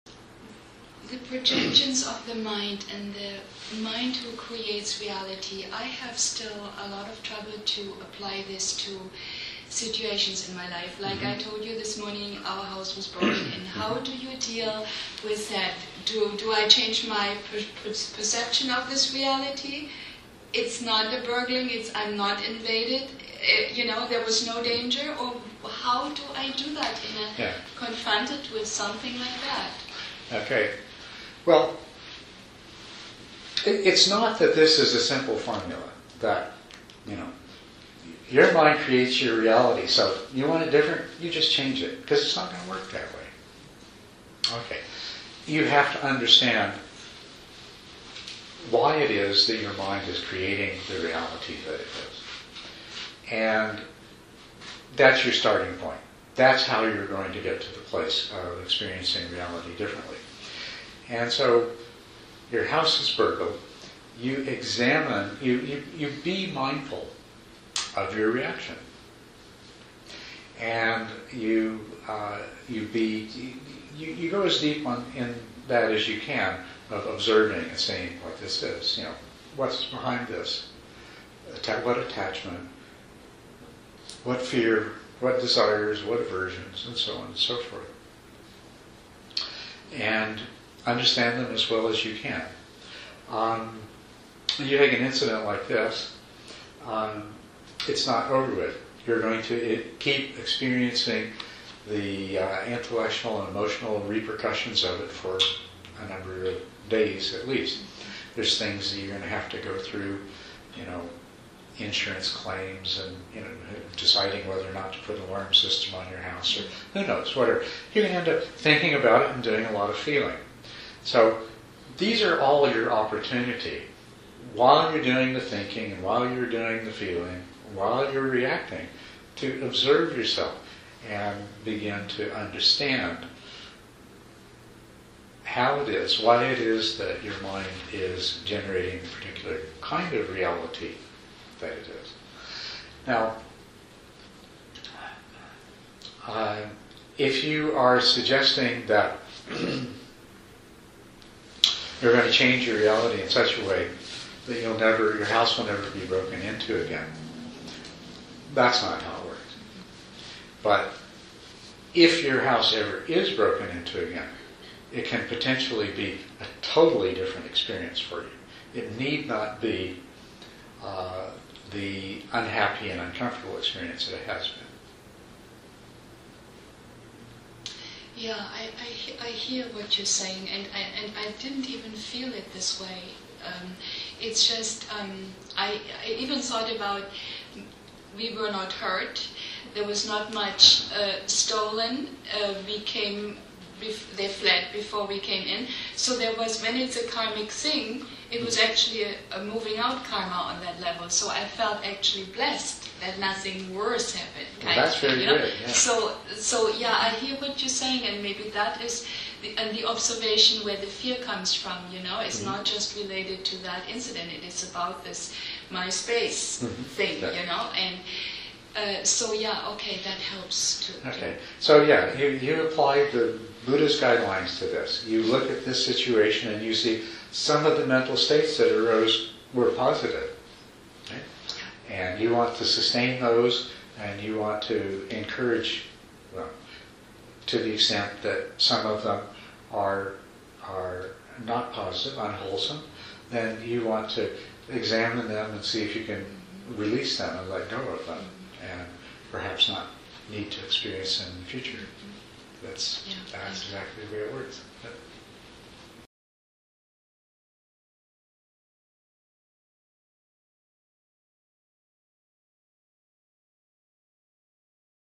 How Do We Project Reality? - Culadasa
How do we project reality?
Master Culadasa discusses how we can change reality in regards to an event like being robbed.